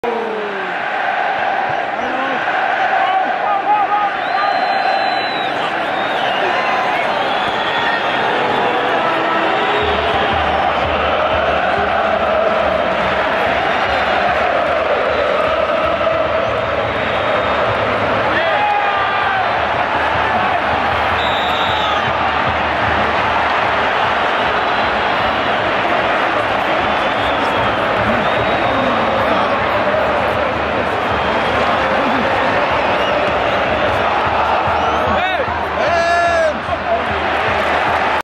【超高画質】素晴らしい守備をした時のサポーターの声援が最高
8月はスペインでの映像を毎日投稿